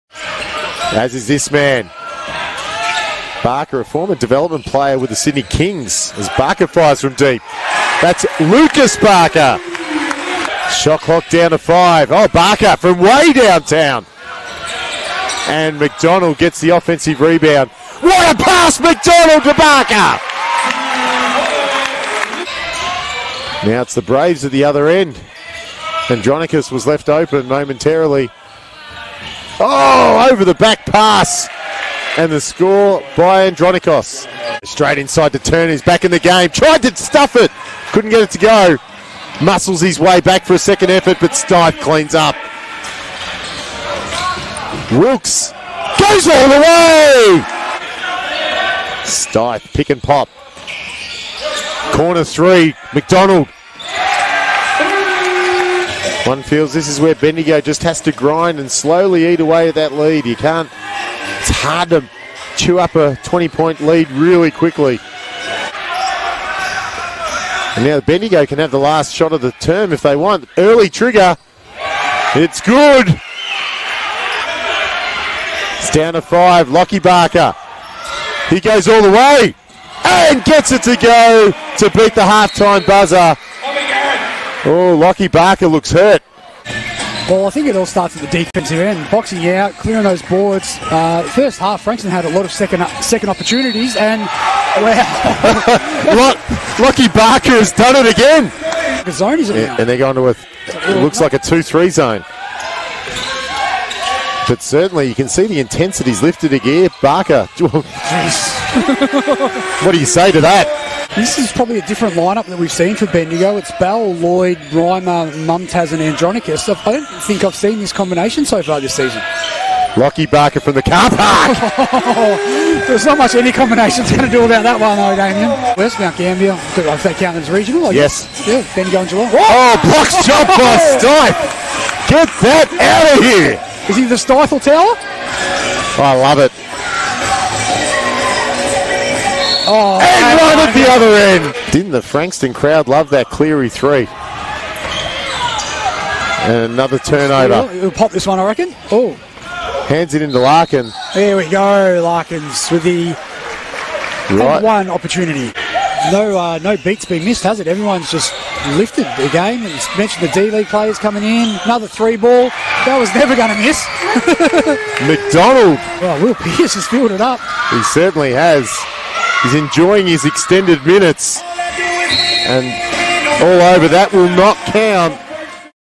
Frankston Blues vs. Bendigo Braves - Game Highlights
Watch the Game Highlights from Frankston Blues vs. Bendigo Braves, 05/16/2021